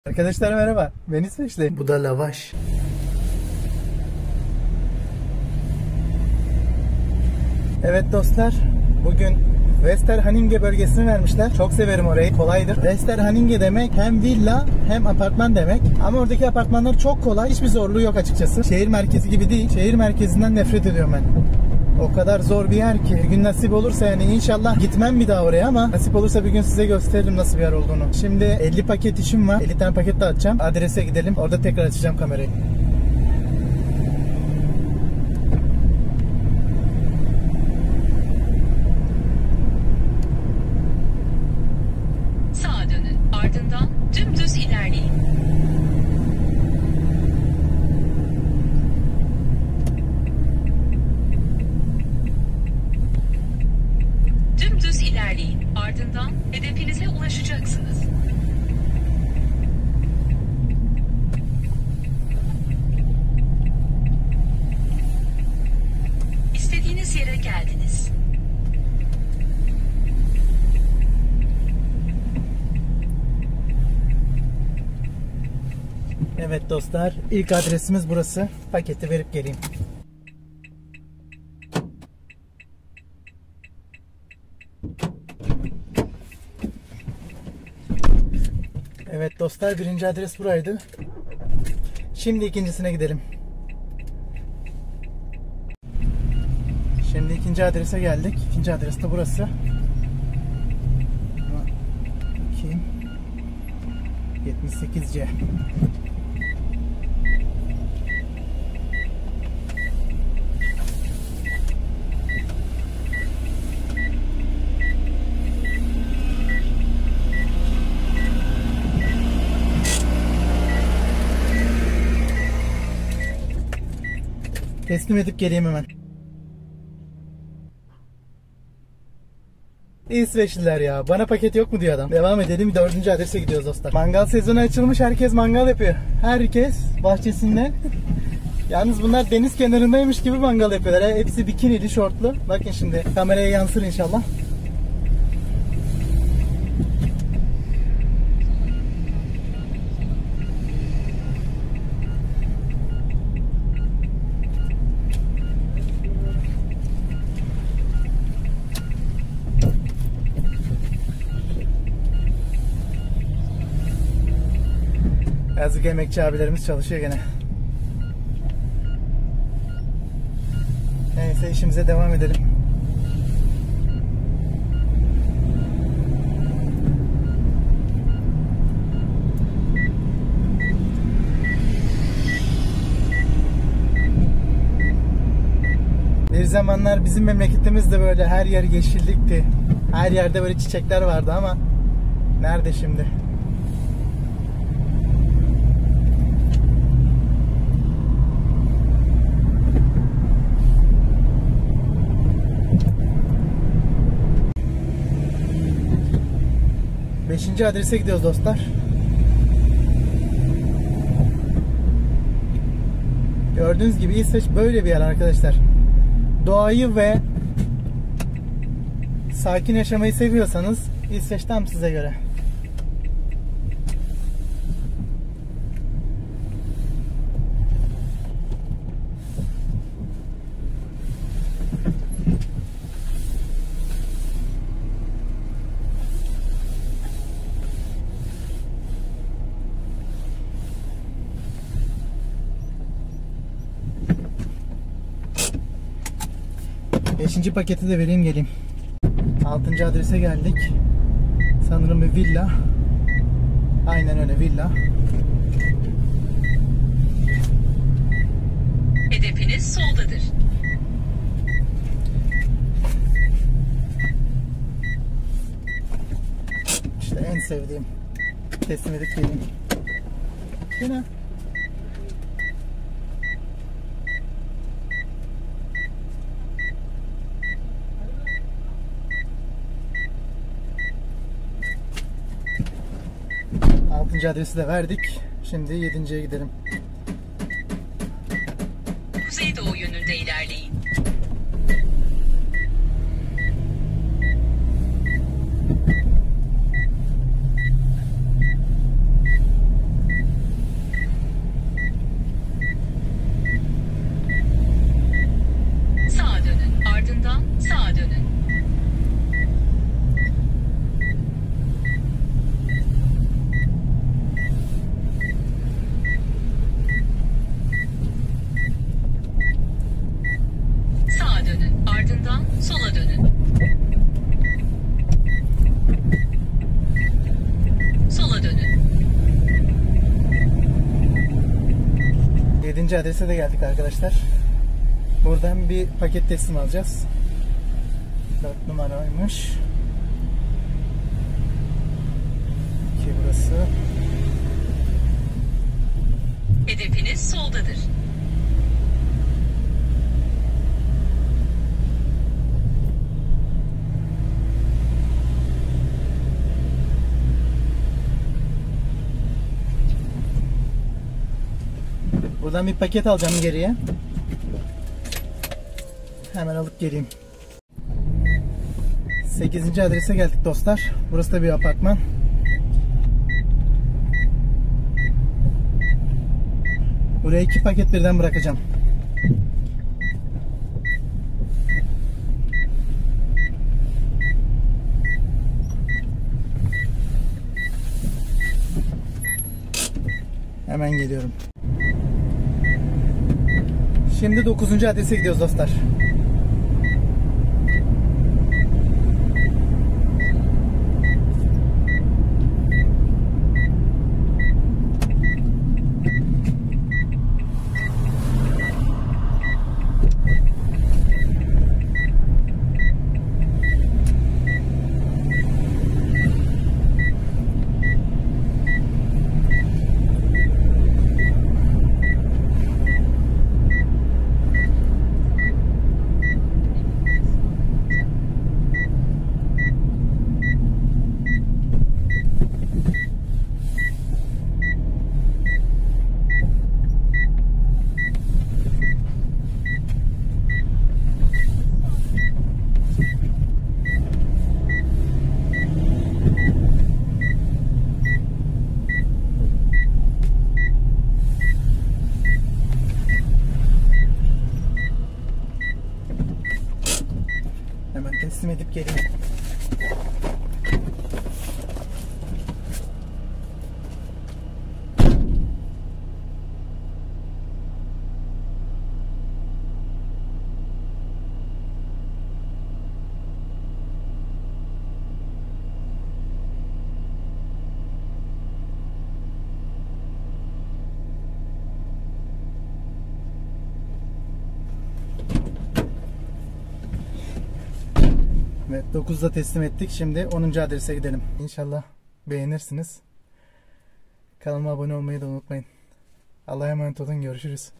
Stockholm'de 1 Mesai Günüm (Part 1)
Merhaba! Bugün sizlerle Stockholm'deki mesai günümün nasıl geçtiğini paylaşacağım. 🚀 İsveç'in başkentinde bir iş gününün nasıl olduğunu merak ediyorsanız, doğru yerdesiniz! 🤓

Bu videoda neler bulacaksınız:

Ofise gidiş ve şehir manzaraları 🏢
İsveç iş kültürü hakkında ipuçları 💡
Akşam işten sonra Stockholm sokaklarında kısa bir tur 🚶‍♂️